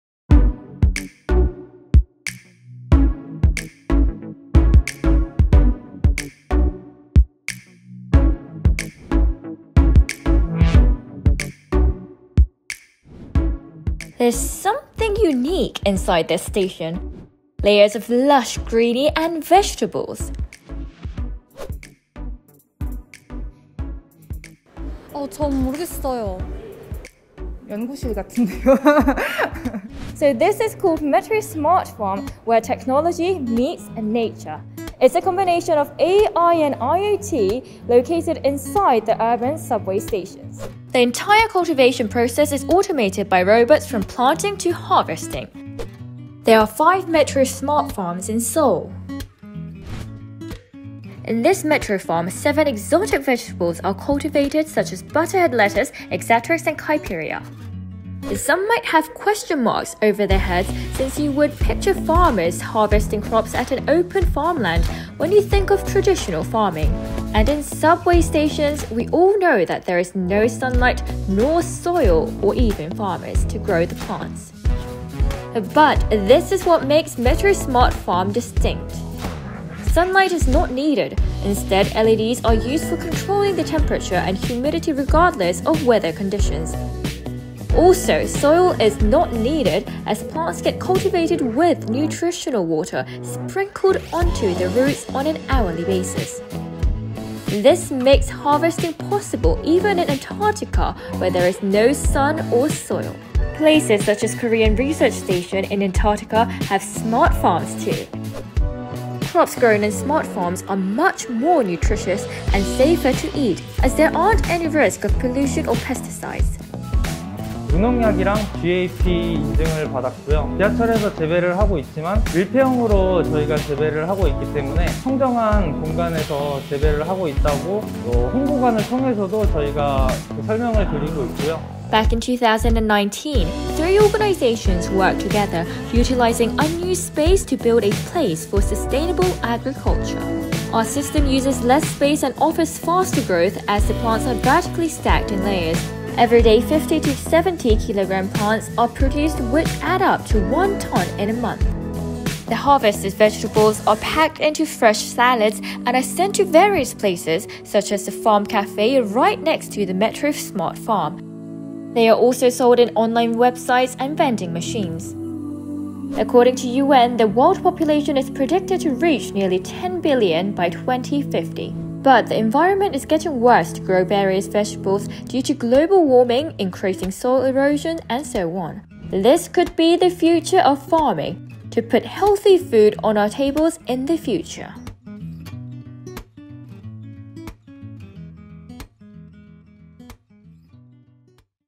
The 'Future of farming', Metro Smart Farm
There is something unique inside this station. Layers of lush greenery and vegetables. This place is the so-called ‘Metro Smart Farm’,  a place where technology meets nature! Come and see the future of farming